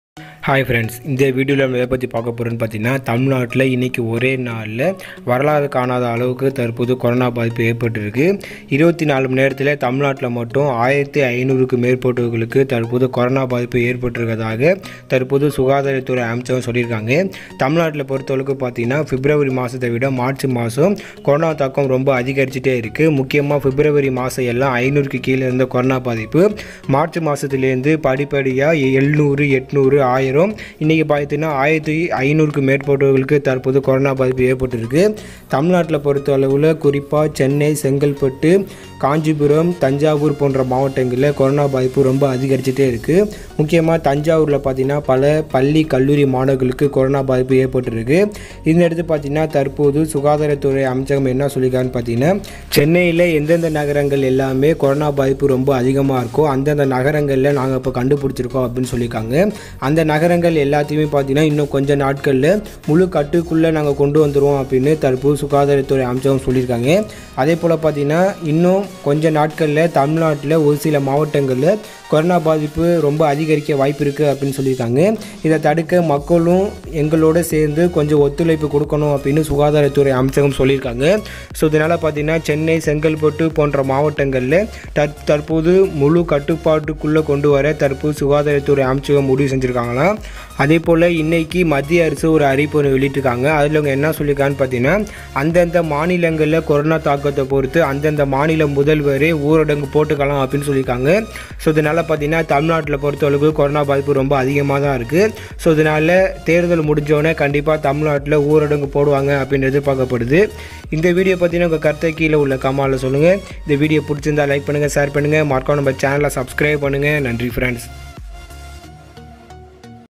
தமிழகத்தில் வரலாறு காணாத அளவுக்கு கொரோனா அதிகரிப்பு! அழிவின் விளிம்பில் தமிழகம்! | Corona Update
தமிழகத்தில் வரலாறு காணாத அளவுக்கு கொரோனா அதிகரிப்பு! அழிவின் விளிம்பில் தமிழகம்! | Corona Update | Tamilisam |